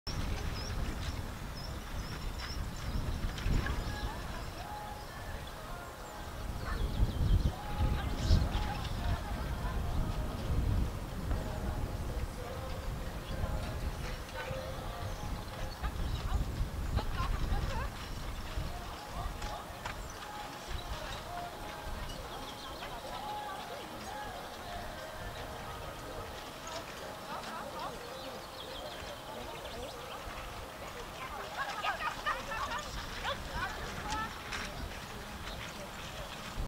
Fahrturnier Holdorf 7.5.2023
Ein kurzes Video einer Marathon-Fahr-Prüfung vom Fahrturnier in Holdorf